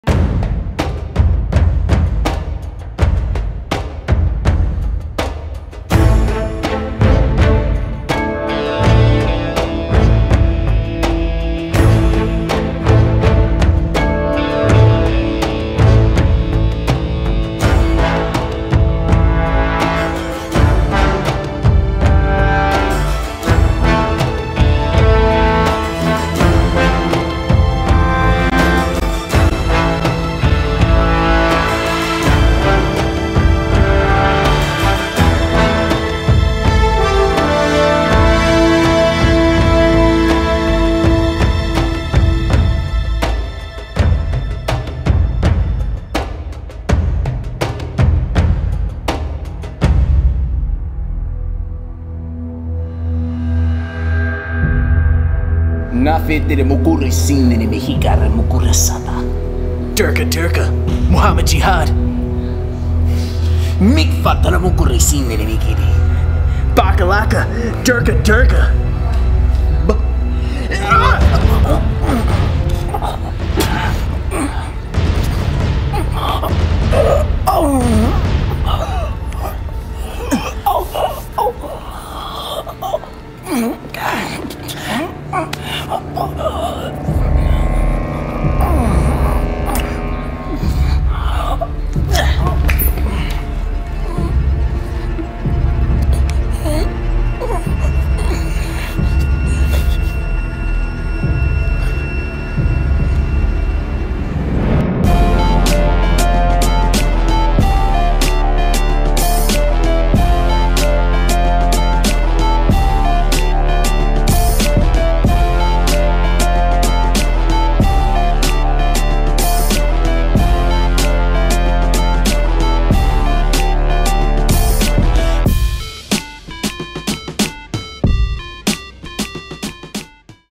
Derka Derka
Watch what happens when Danny talks smack to the wrong person!